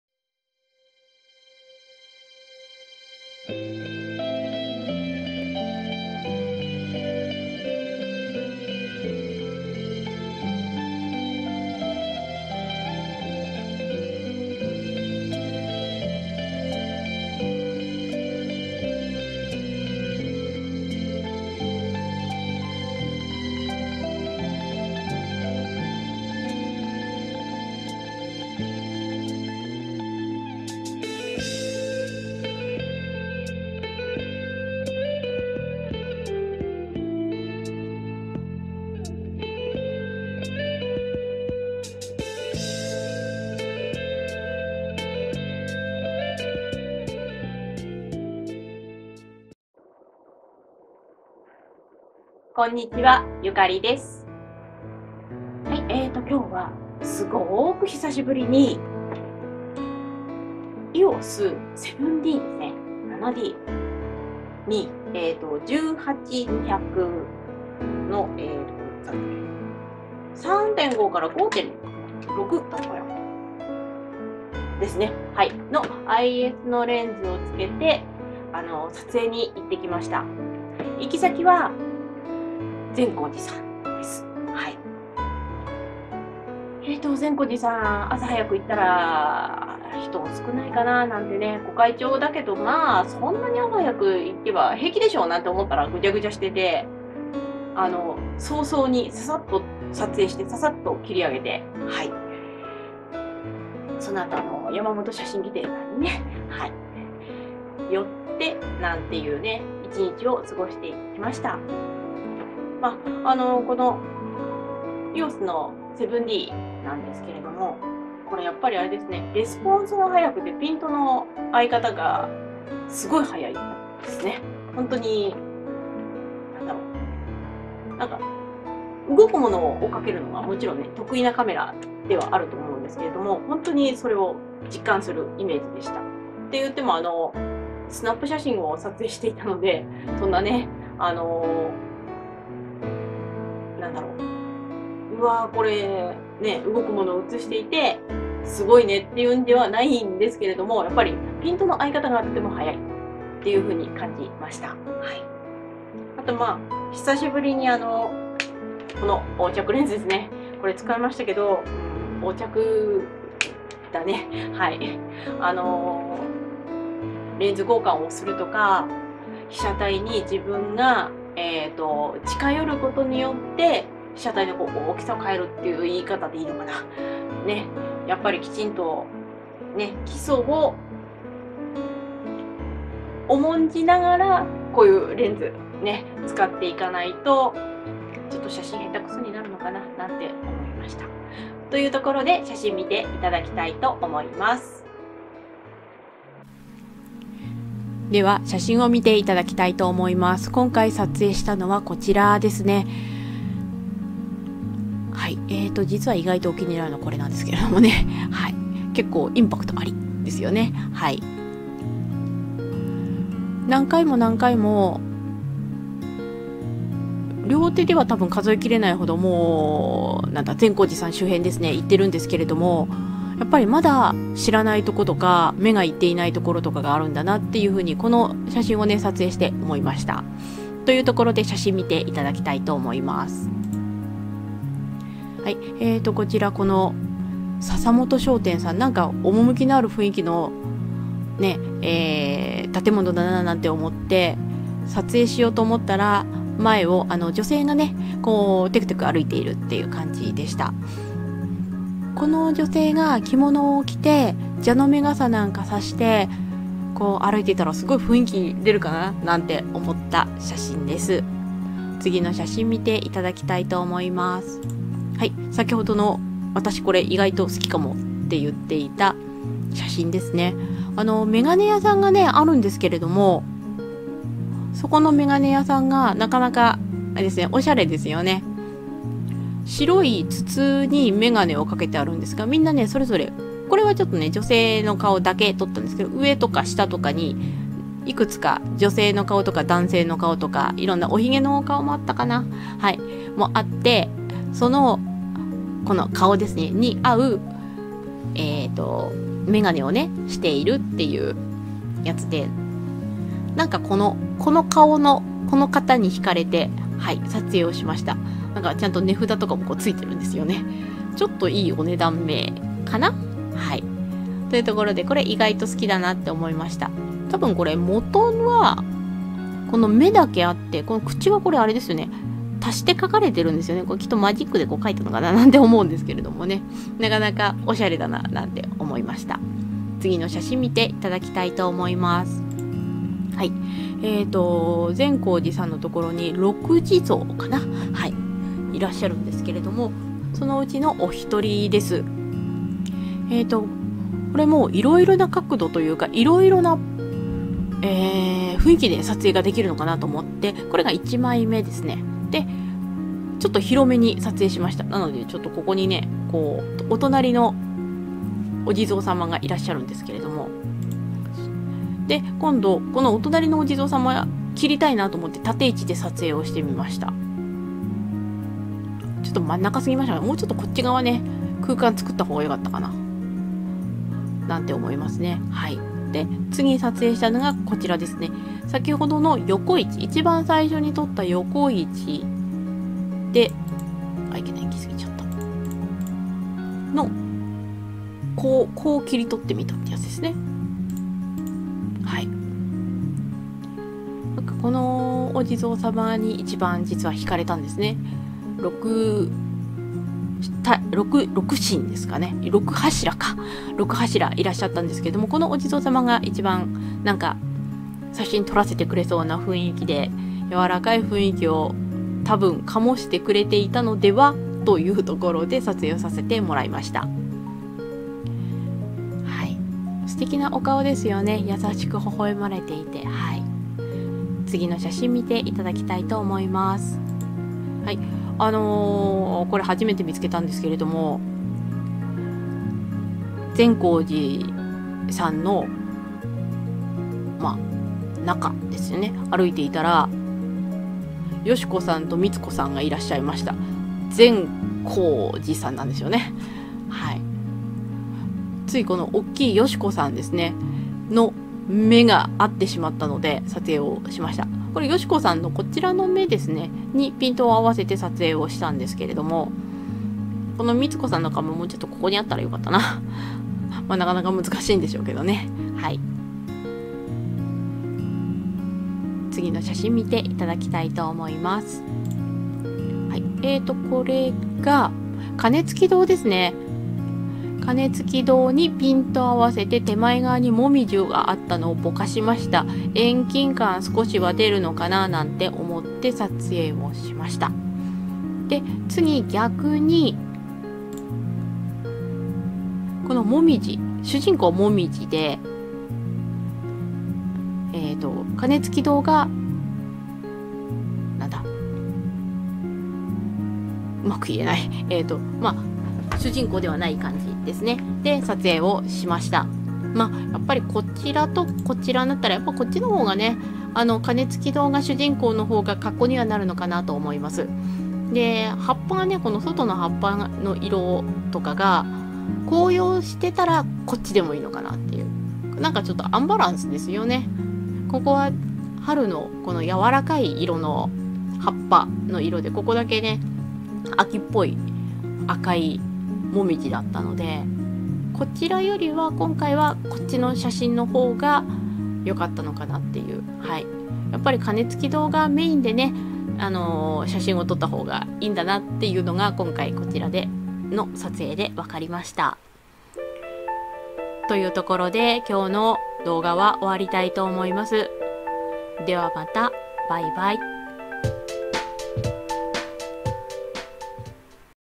御開帳真っ只中の善光寺へ
こんにちは ゆかりです

早朝なら
たくさんの人がいなくて撮影もしやすいだろうな♪
なんて思って朝早く家を出たのに
まだお店の始まる前の時間にもかかわらずたくさんの人が･･･

人のいるところをできるだけ避けて撮影を楽しんできました！